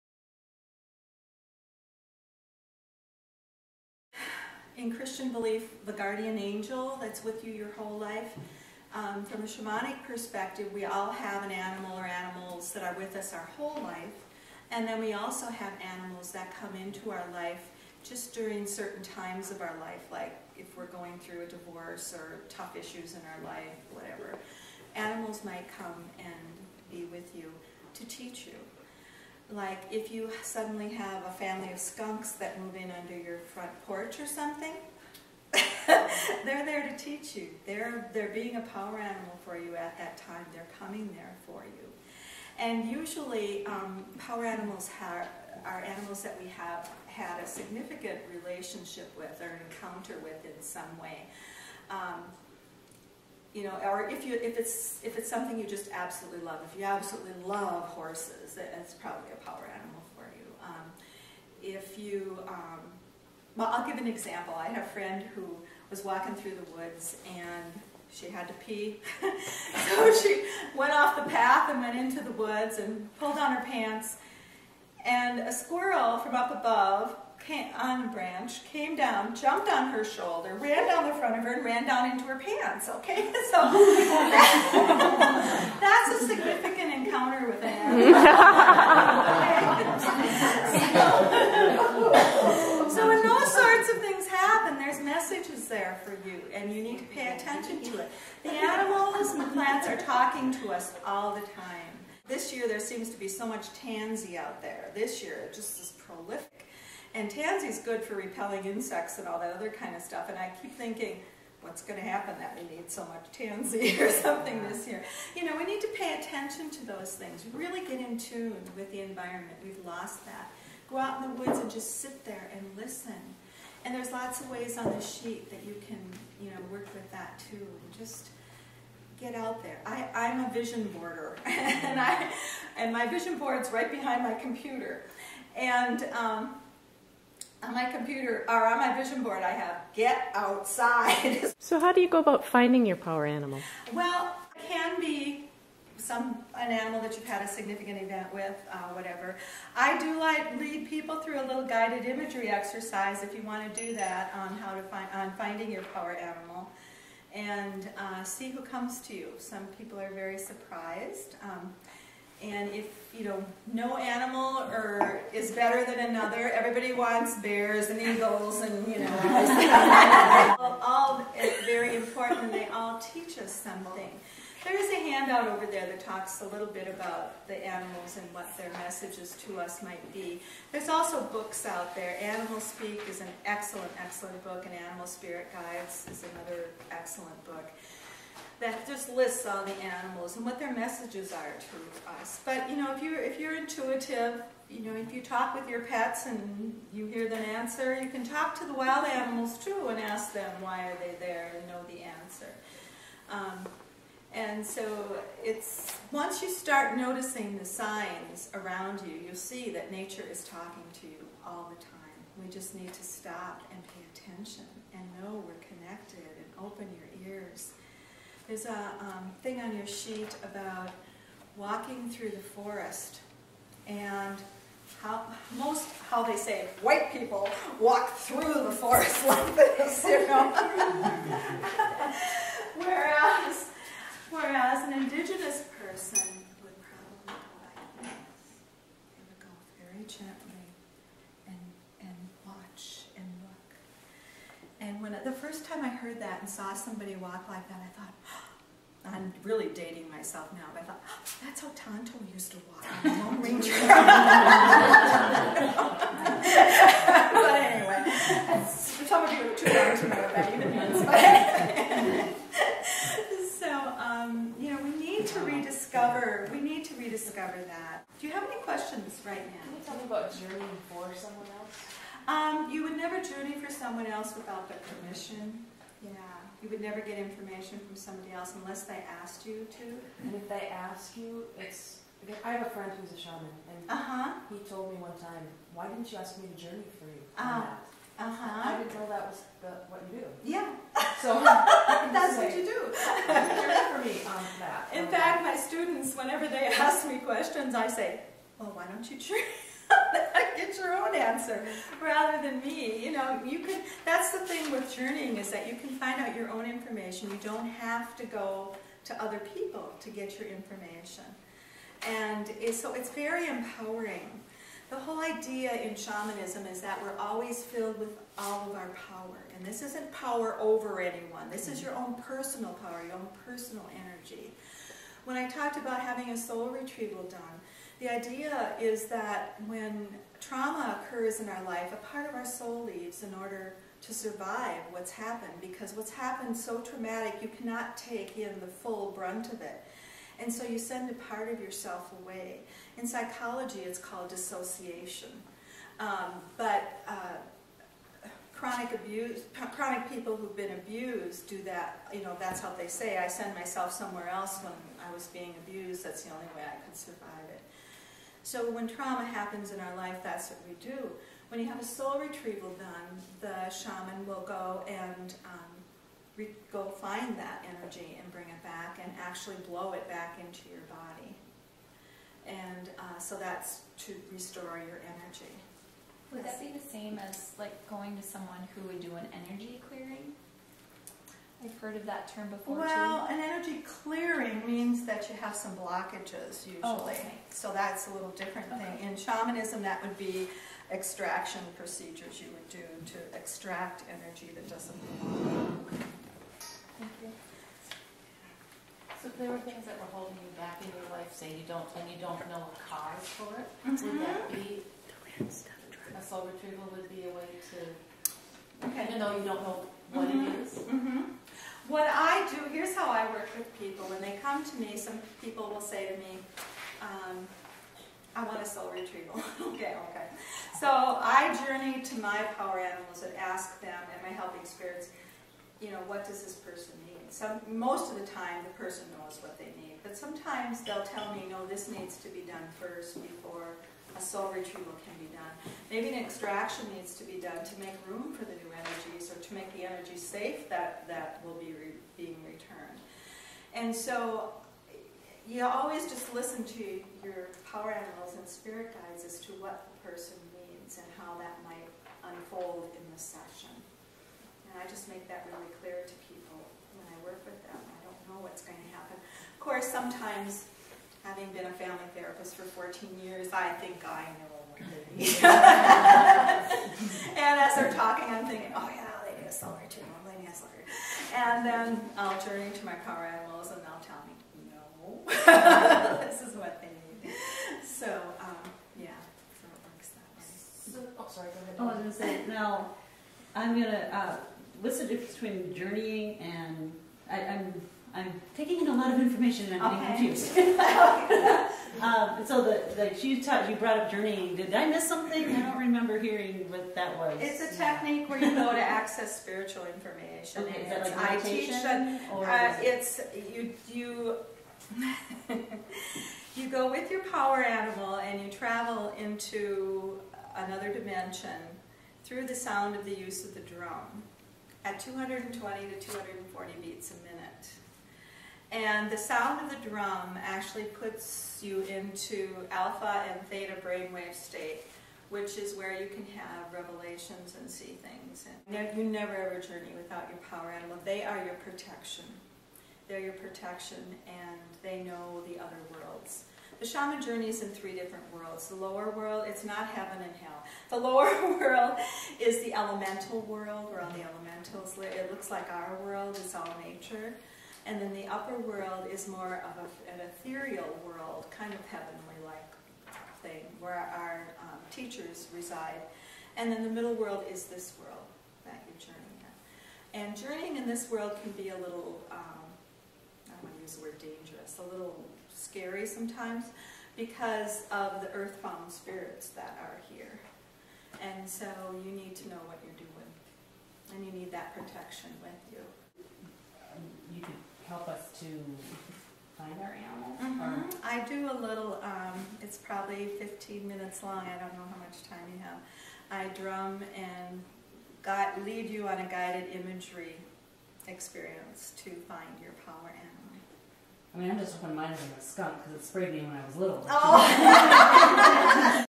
Shamanism Gathering - Part II
Jeanne Marie Troge shares her experience as a shaman and how to find your power animal.
Join women across the globe as we connect, inspire, nurture and support one another.